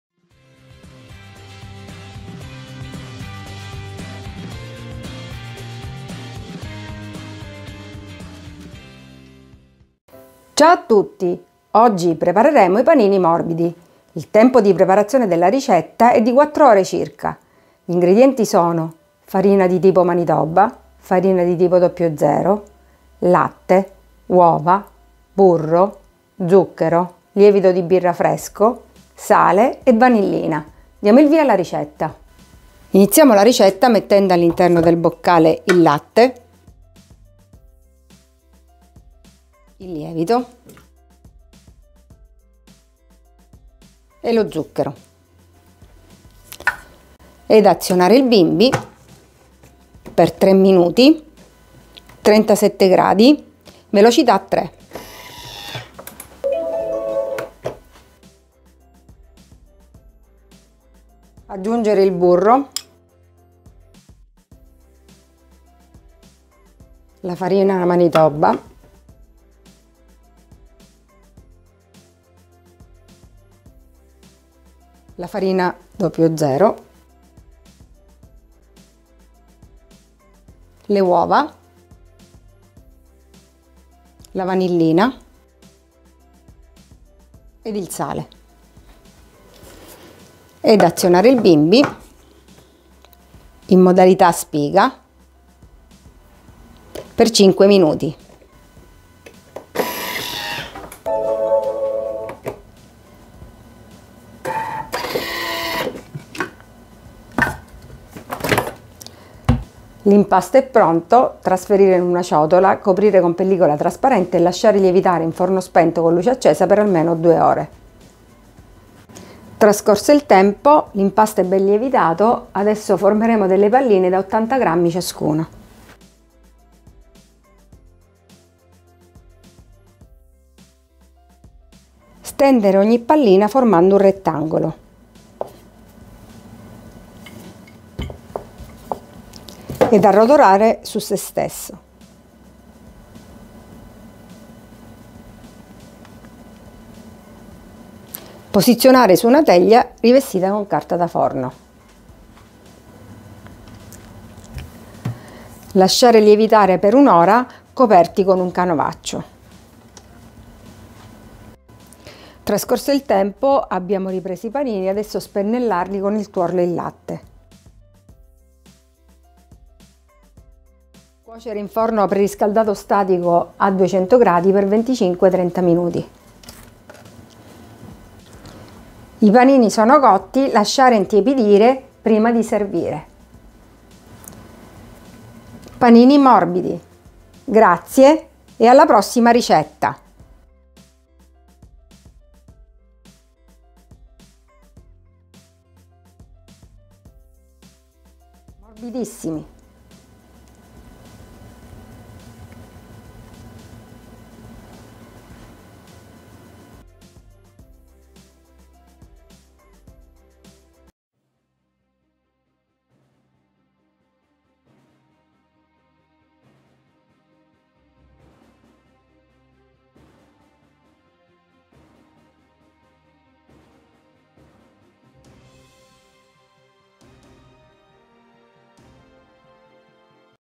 Panini morbidi per bimby TM6 TM5 TM31 TM21
Dosi per 16 panini

Ingredienti:
600 g di farina di tipo manitoba
200 g di farina di tipo "00"
300 g di latte
2 uova
100 g di burro
40 g di zucchero
15 g di lievito di birra fresco
15 g di sale
1 bustina di vanillina